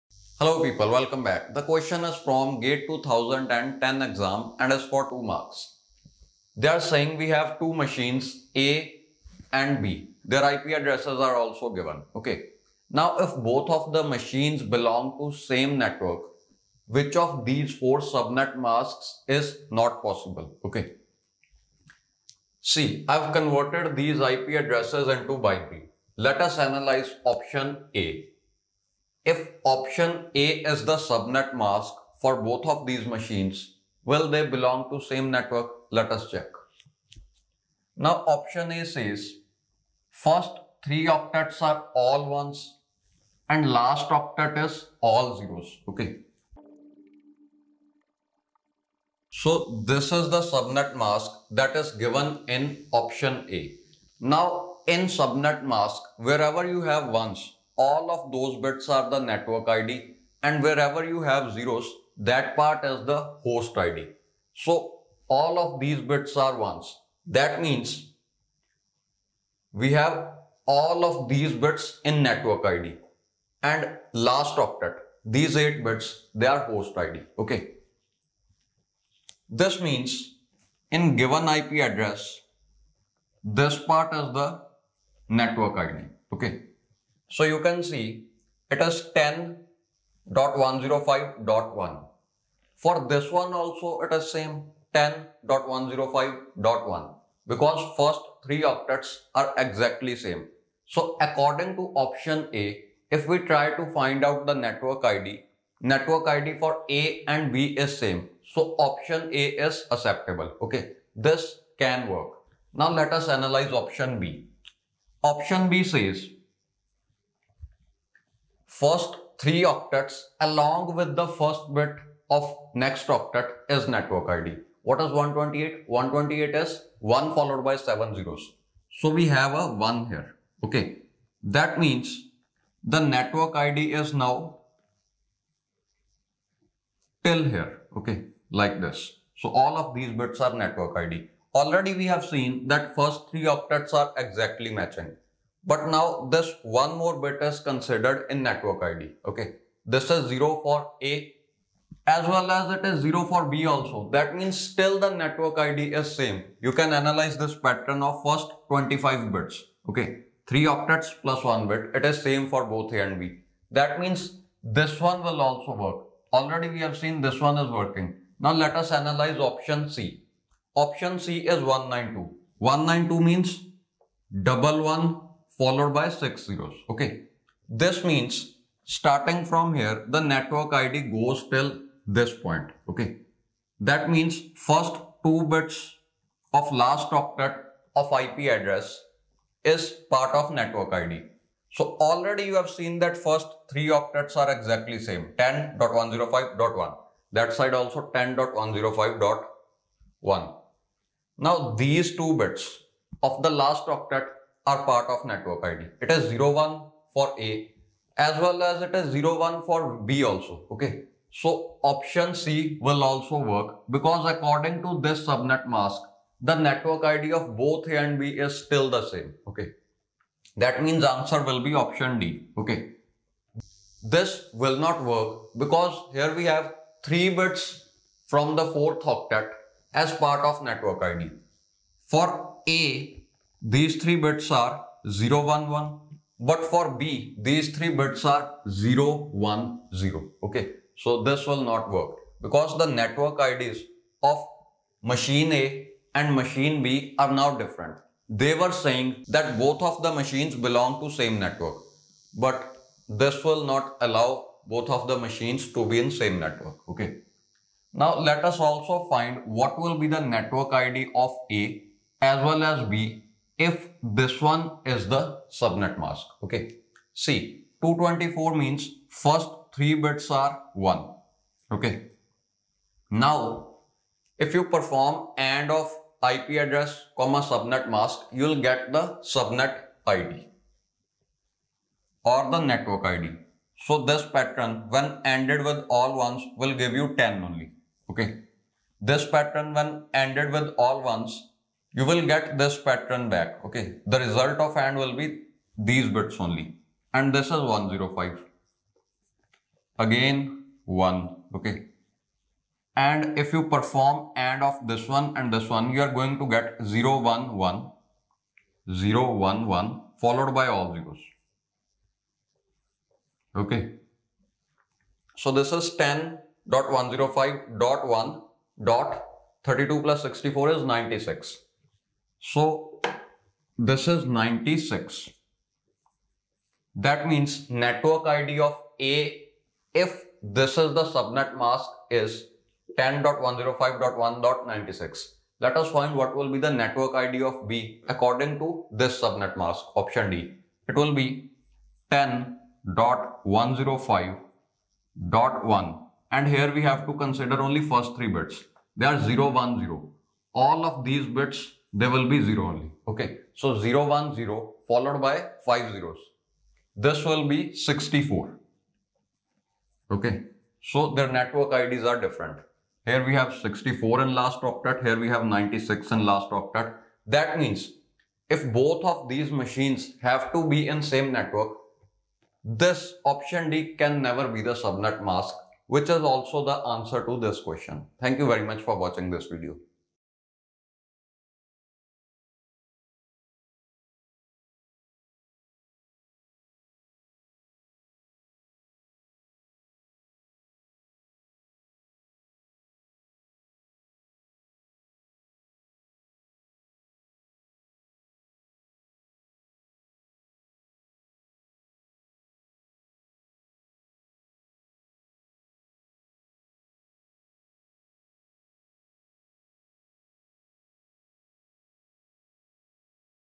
Gate 2010 pyq CN | Suppose computers A and B have IP addresses 10.105.1.113 and 10.105.1.91
Suppose computers A and B have IP addresses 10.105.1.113 and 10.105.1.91 respectively and they both use the same netmask N. Which of the values of N given below should not be used if A and B should belong to the same network?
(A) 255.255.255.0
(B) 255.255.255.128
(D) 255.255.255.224